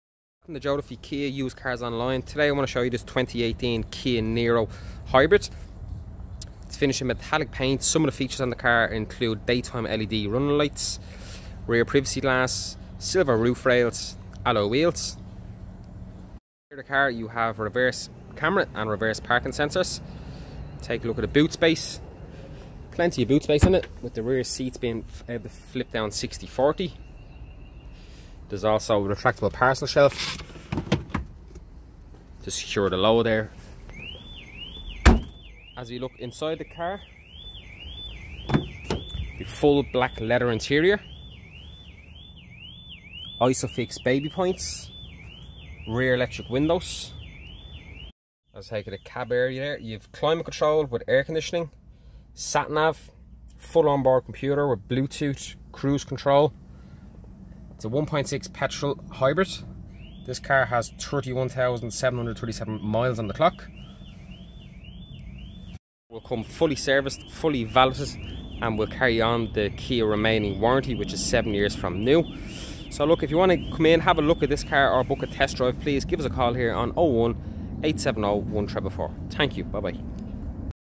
LF18XBW - 2018 Kia Niro Niro 1.6 GDi 104BHP 5dr PHEV 24,995
Here is a video of our LF18XBW - 2018 Kia Niro Niro 1.6 GDi 104BHP 5dr PHEV 24,995. PLF18XBW - 2018 Kia Niro Niro 1.6 GDi 104BHP 5dr PHEV 24,995ease contact sales to arrange a test drive today. Joe Duffy KiaPhone: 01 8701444